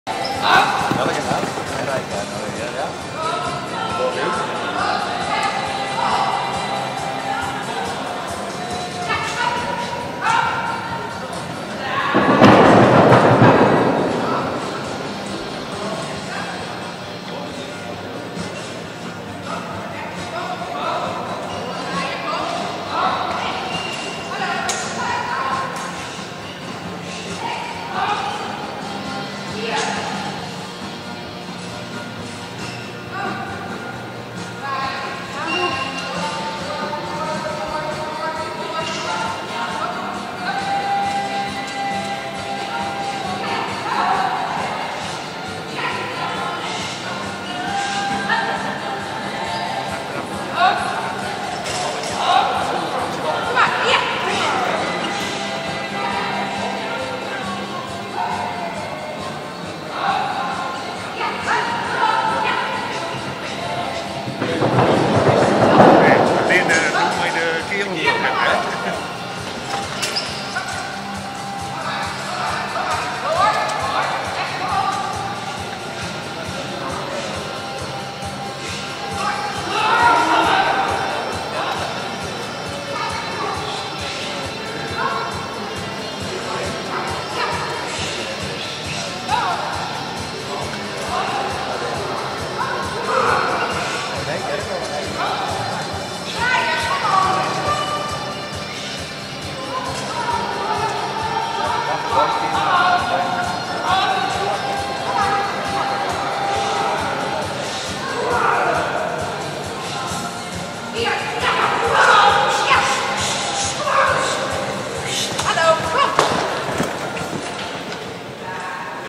Fahrturnier Bathmen 2.3.2024
Ein kurzes Video einer Kegel-Fahr-Prüfung mit festen Gelände Hindernissen vom Fahrturnier in Bathmen
Indoor Turnier
Wegen Urheberrecht sind die Videos teilweise ohne Ton
Bilder von verschiedenen Turniergelände bei Instagram:
Pferde.Flo